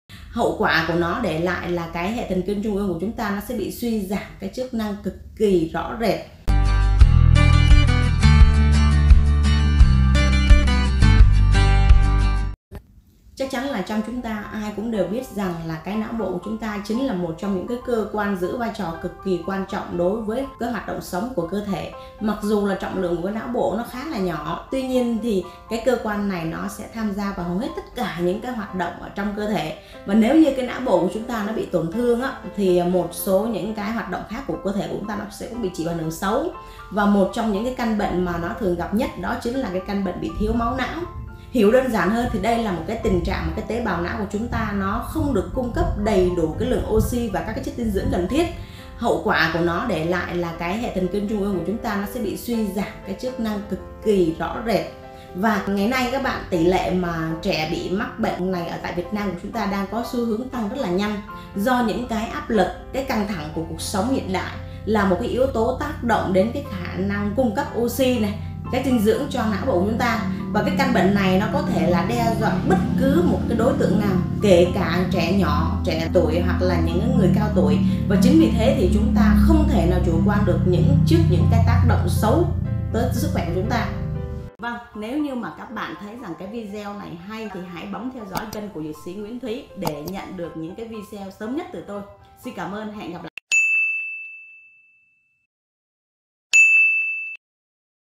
Bệnh thiếu máu não - căn bệnh ảnh hưởng lớn đến sức khỏe I Dược sĩ Nguyễn Thúy
Não bộ đóng 1 vai trò vô cùng quan trọng, nó tham gia vào hầu hết các hoạt động sống trong cơ thể con người. Nếu não bộ bị tổn thương thì đây là 1 điều đáng báo động, điển hình là bệnh Thiếu Máu Não sẽ làm suy giảm chức năng hệ thần kinh, chính bởi việc không cung cấp đủ Oxi và chất dinh dưỡng cần thiết cho não. Tỷ lệ người trẻ mắc phải căn bệnh này ngày càng gia tăng bởi stress và áp lực từ cuộc sống. Từ những tác động xấu đấy bạn không nên chủ quan với căn bệnh nguy hiểm này.
👉 Video liên quan: Triệu chứng của bệnh nhân thiếu máu não

Đăng kí tình nguyện viên hoặc Nhà phân phối :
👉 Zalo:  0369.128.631